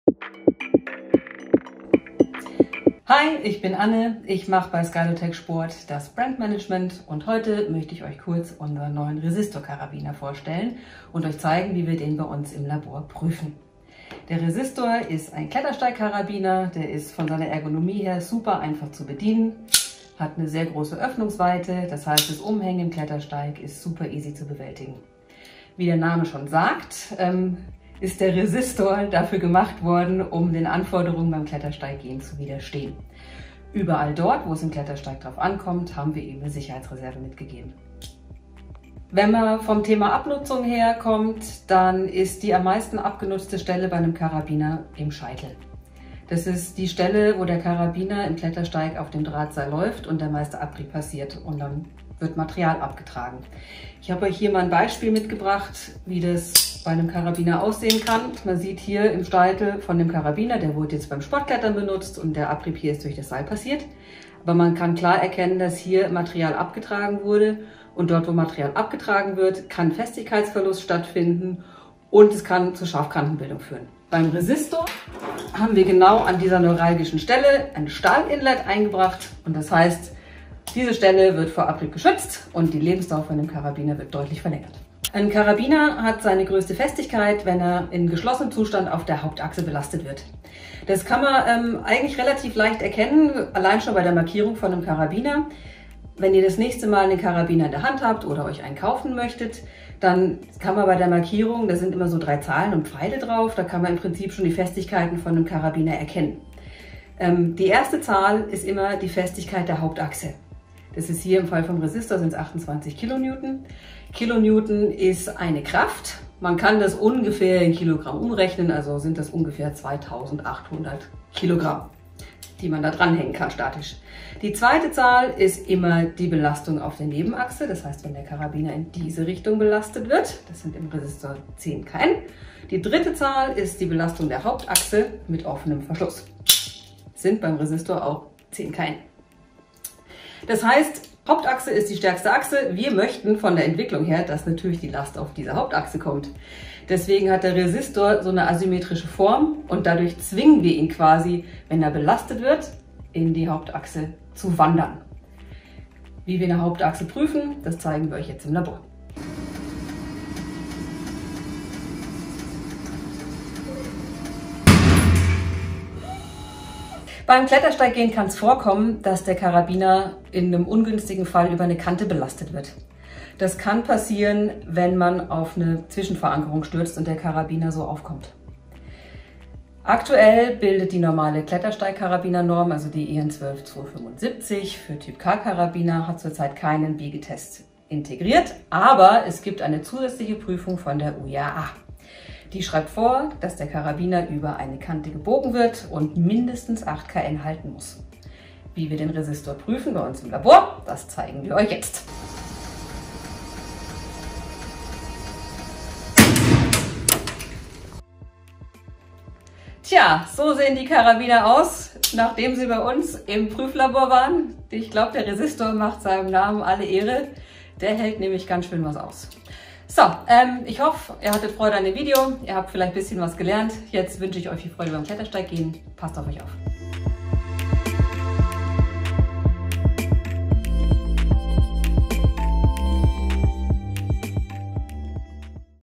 SKYLOTEC - RESISTOR CARABINER EXPLANATORY VIDEO
Thanks to its intelligent design, the new RESISTOR is one of the most innovative and safest via ferrata carabiners on the market. Its geometry and ergonomics are perfectly adapted to the requirements of via ferrata:

- The steel inlet in the crown protects against abrasion
- Large opening width for easy insertion and removal
- Can be easily operated with small hands due to the narrow diameter
- Withstands a load even over edges of 13 kN